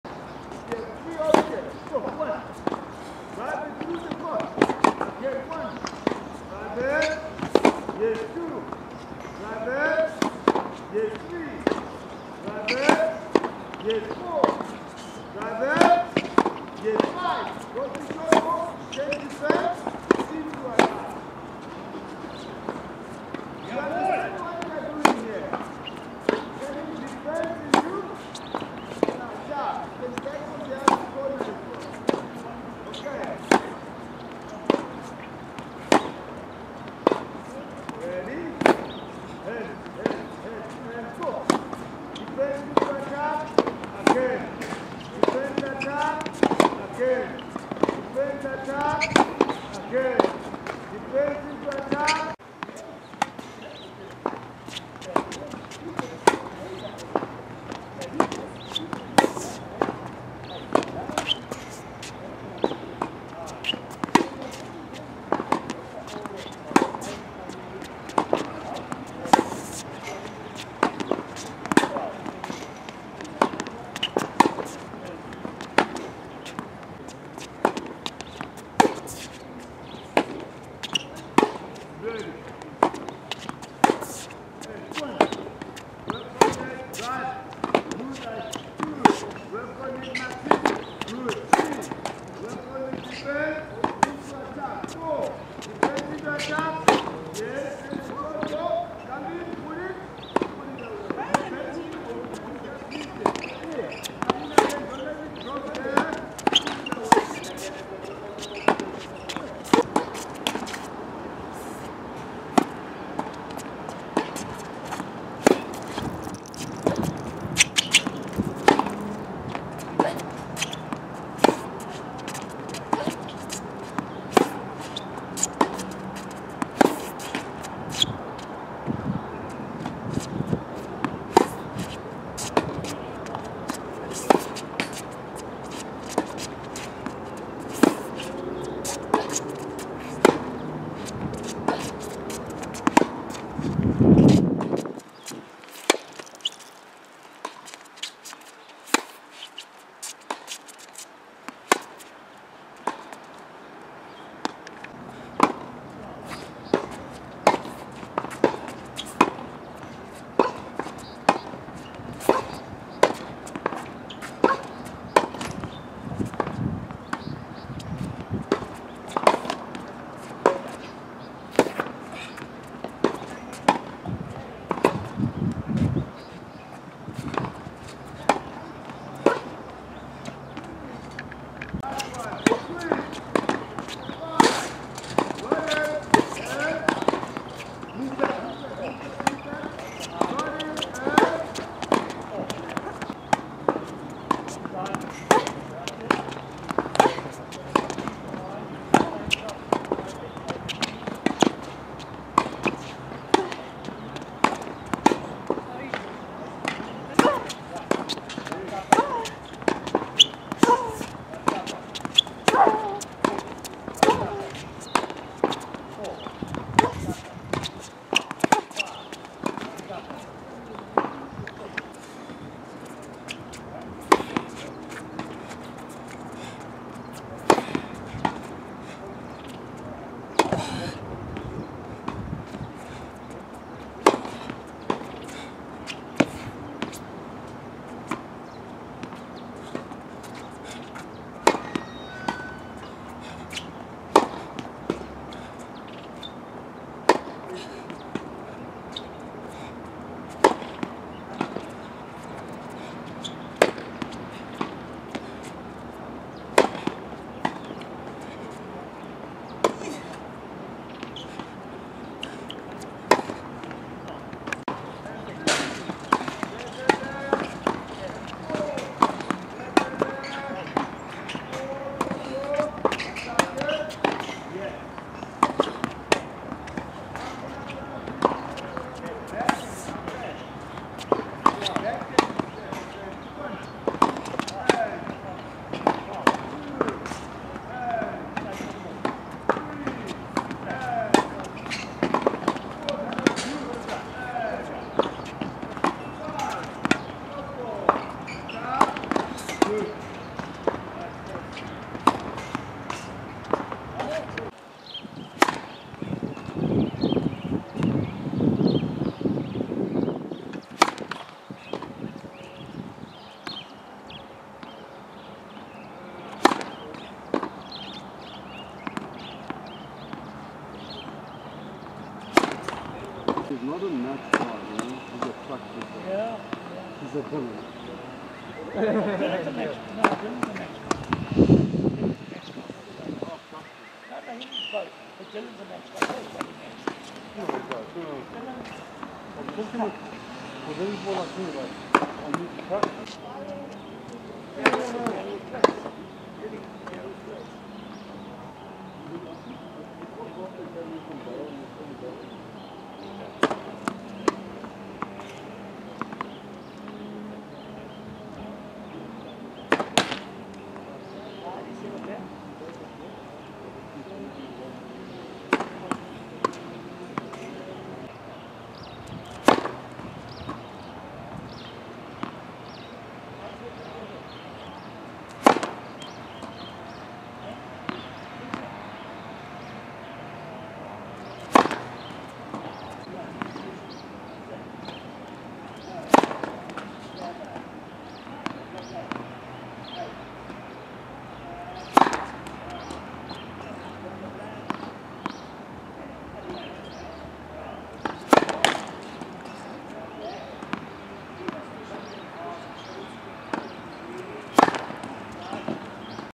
Mbali Langa Tennis Shots
Mbali Langa's college tennis video
I am looking for college scholarship for August 2020